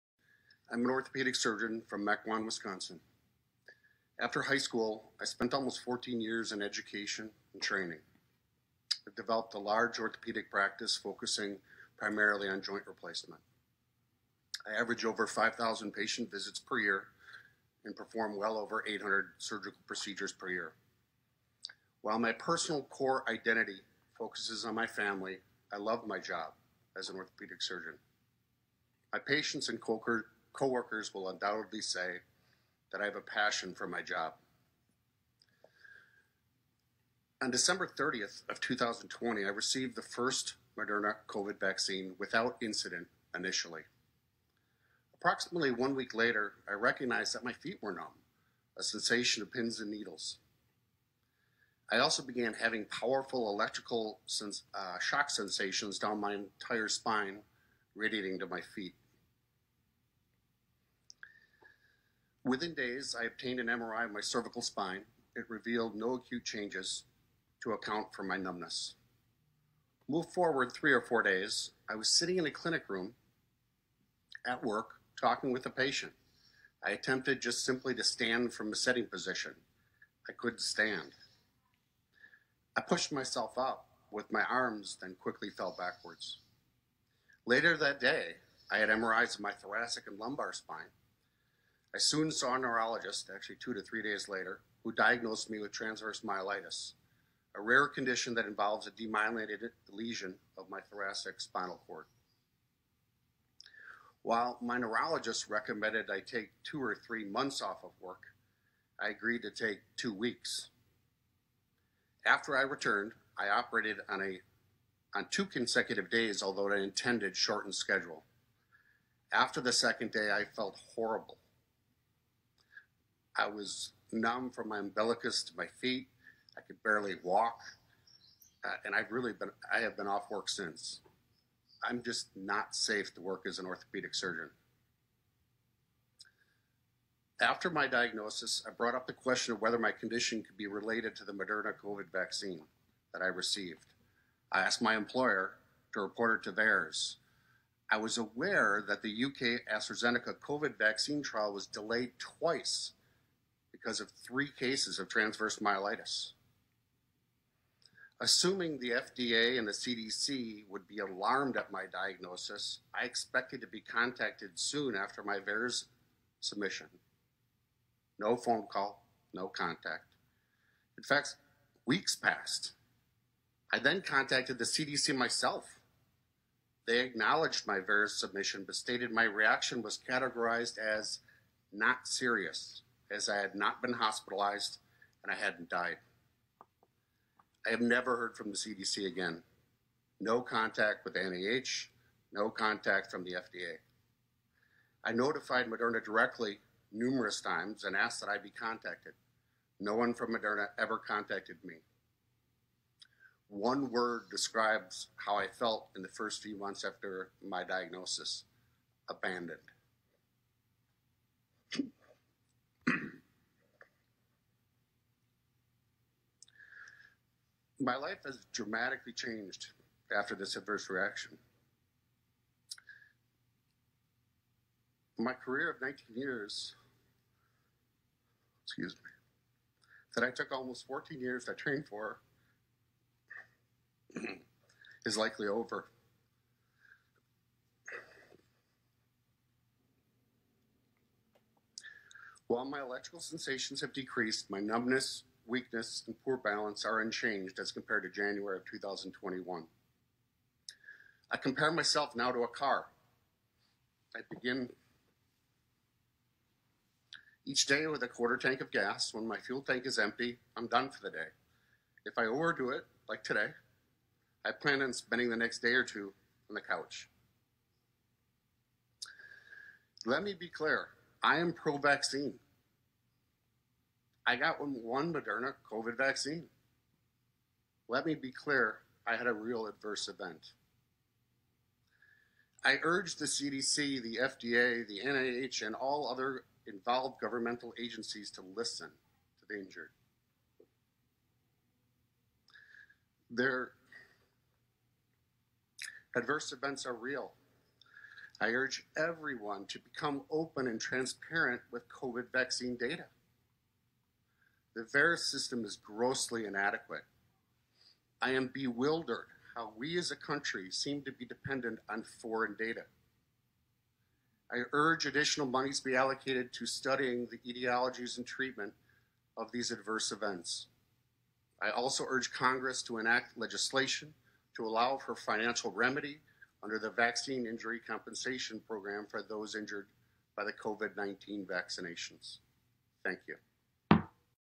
DESGARRADOR TESTIMONIO DEL CIRUJANO ORTOPÉDICO, Dr. JOEL WALLSKOG, ANTE EL SENADO DE LOS EEUU.
Esperando a ver si me cierran el canal permanentemente.
Fue un placer haber compartido con ustedes, y si sigue abierto, que Dios me guíe en cada asunto que se comparta.